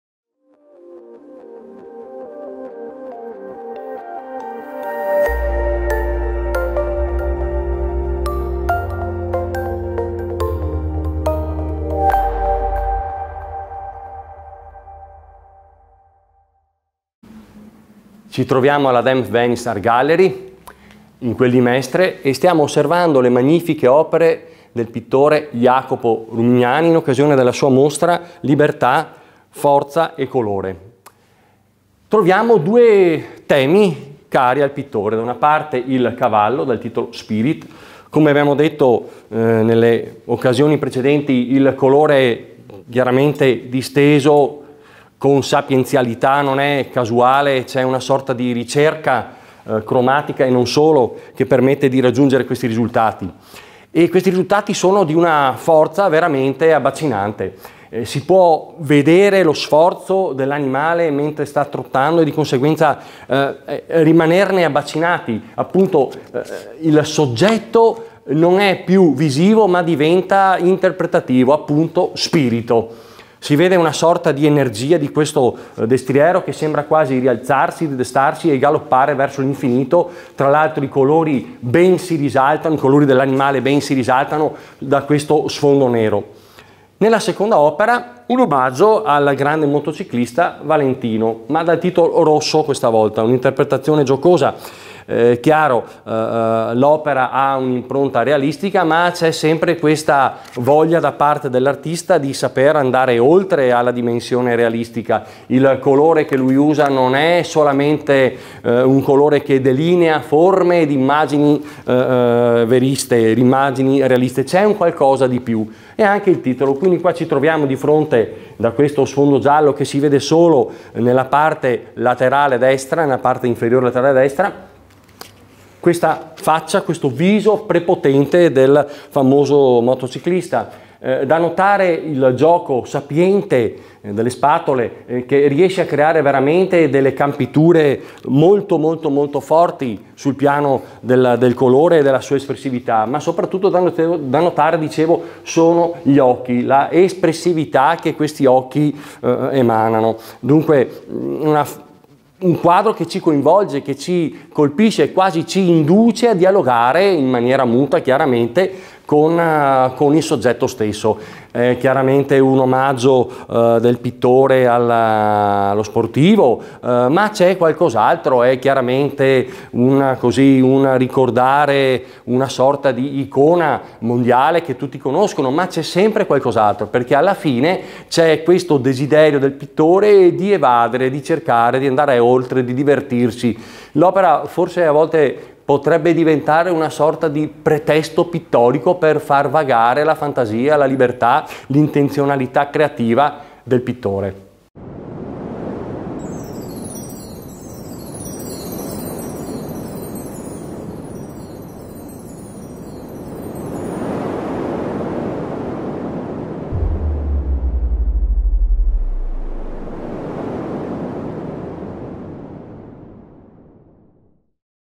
Jacopo Rumignani - Presentazione di Siro Perin III
Il critico e storico dell'arte Siro Perin presenta alcune opere esposte nella mostra "Libertà, Forza, Colore" di Jacopo Rumignani.

The critic and art historian Siro Perin presents some works exhibited in the exhibition "Libertà, Forza, Colore" by Jacopo Rumignani.